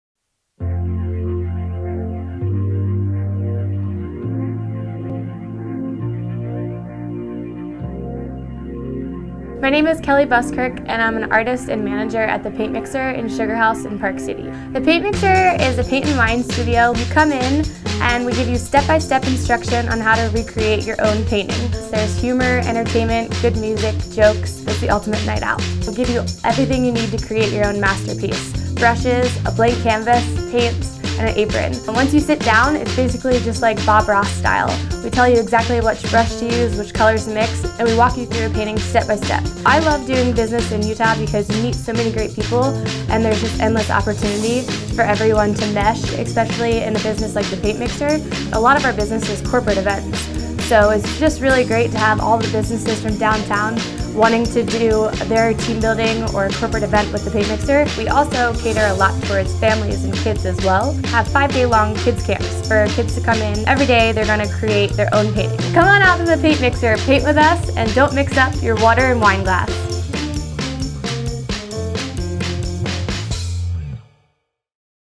Member Spotlight: The Paint Mixer
The Paint Mixer offers a unique mix of art and fun for all ages and requires no prior art experience or artistic ability. Guests follow a 2-hour step-by-step instruction to recreate the featured piece of art.

They are also our Member Business Spotlight for the month of March.